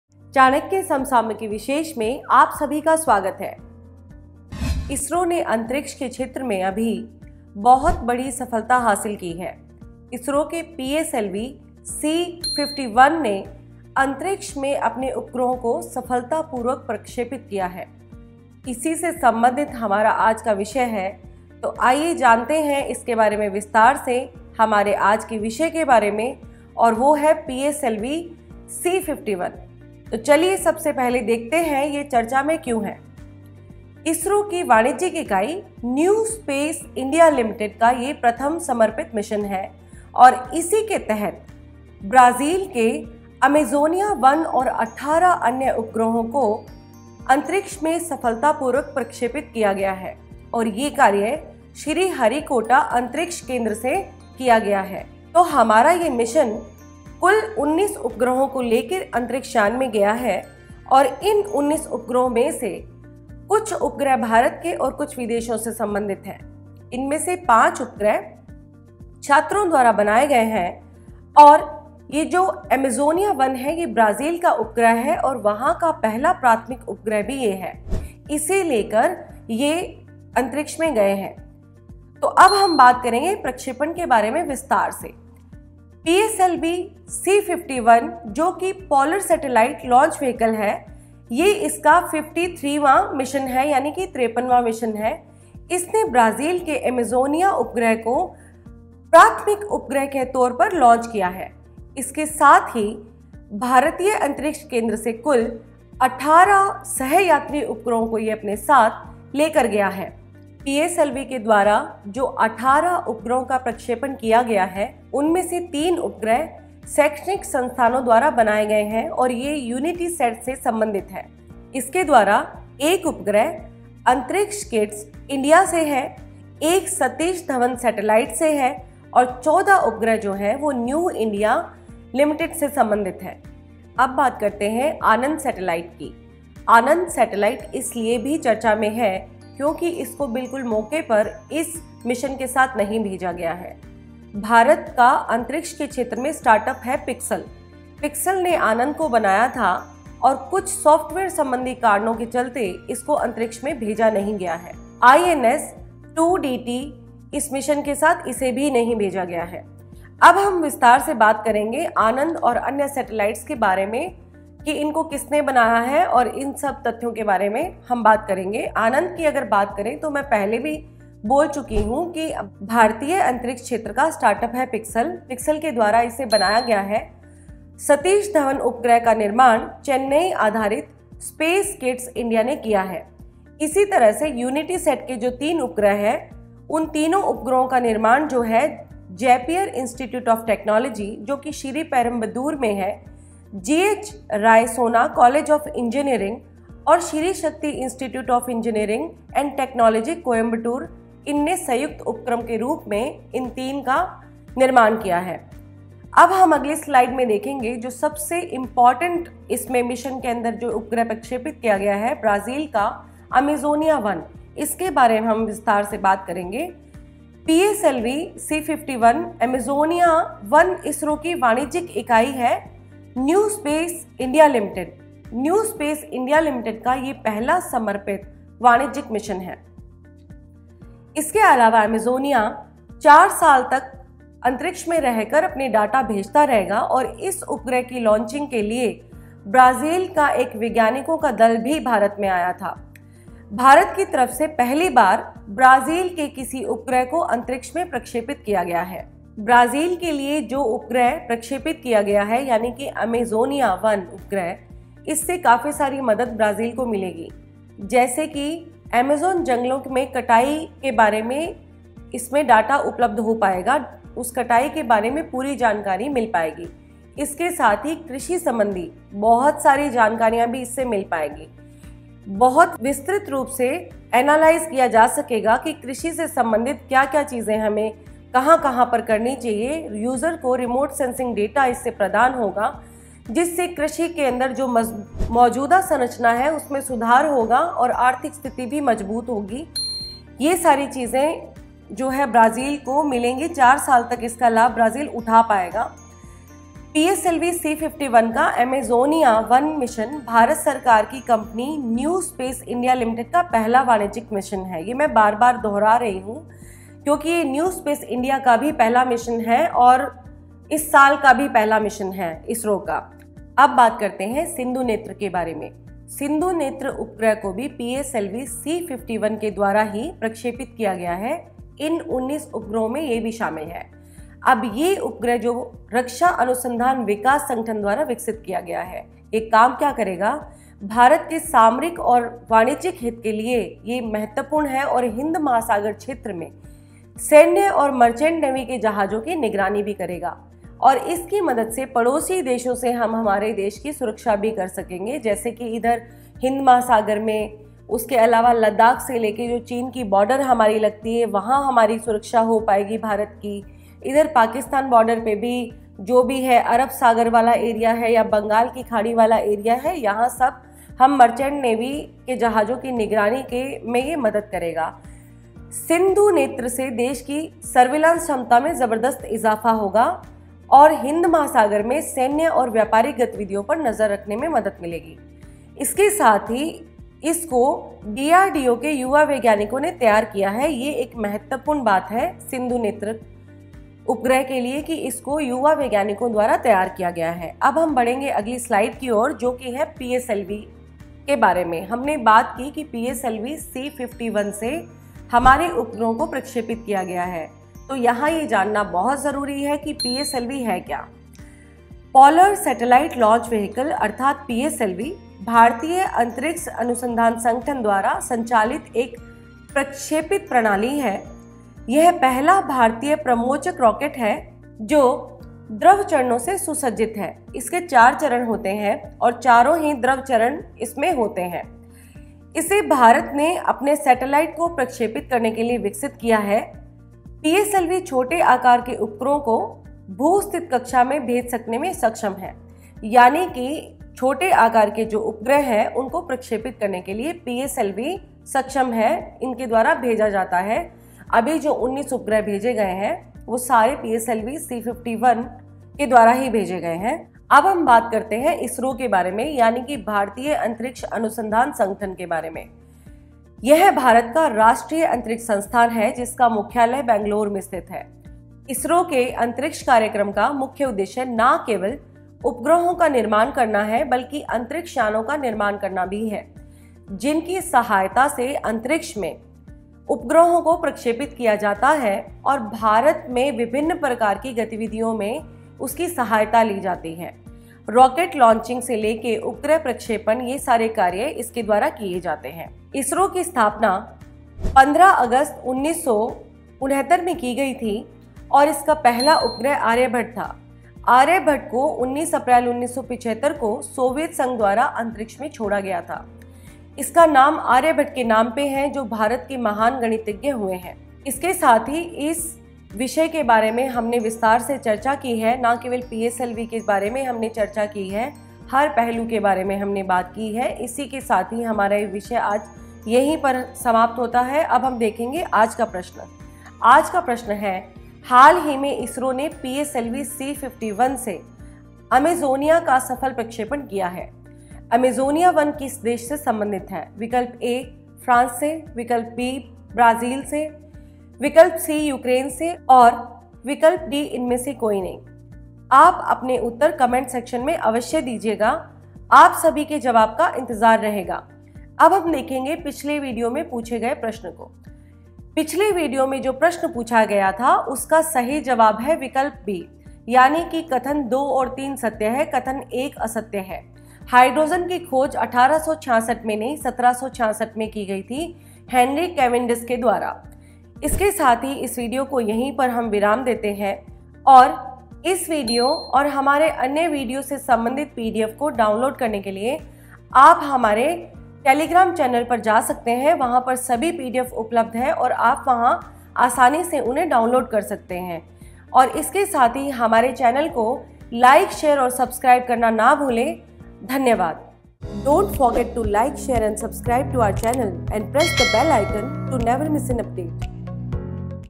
PSLV-C51/Amazonia-1 - ISRO Mission | Current Affairs Today | 3 March 2021 | Chanakya Academy
Daily Current Affairs Today's Topic is PSLV-C51/Amazonia-1 - ISRO. It is the 53rd mission of the Indian Polar Satellite Launch Vehicle (PSLV) program. The Polar Satellite Launch Vehicle (PSLV)-C51 was launched on 28 February 2021 with the main payload from Brazil, INPE's Amazônia-1 and 18 other ride-sharing small satellites.